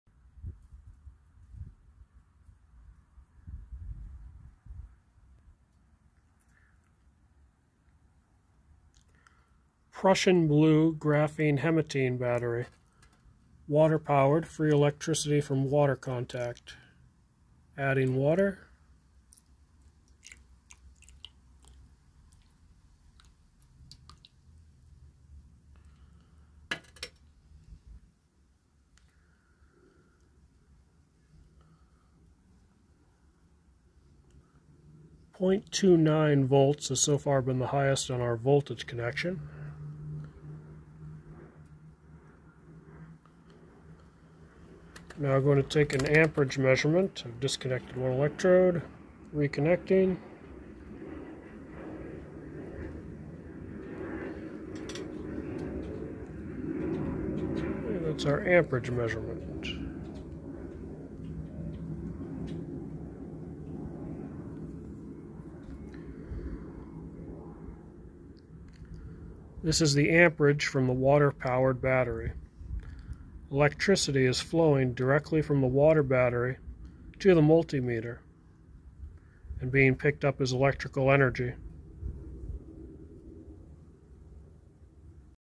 PRUSSIAN BLUE WATER BATTERY
Prussian blue. nanoionic wood, graphene., hematene make free electricity from water with nanotechnology using the electrokinetic streaming effect